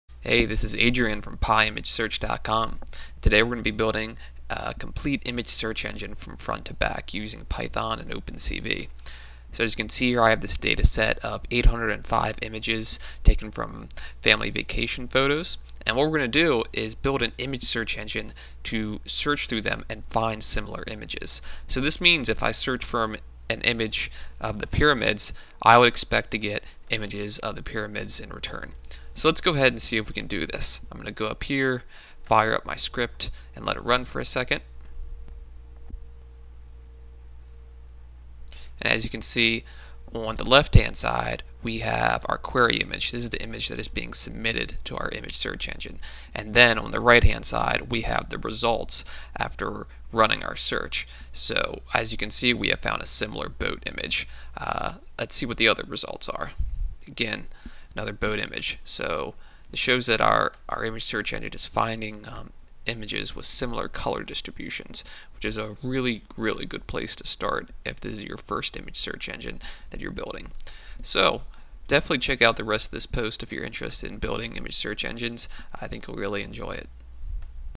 Building an Image Search Engine/Content-Based Image Retrieval System using Python and OpenCV
In this post I'll show you how to utilize OpenCV, Python, and region-based color histograms to build an image search engine, also called a Content-Based Image Retrieval (CBIR) system.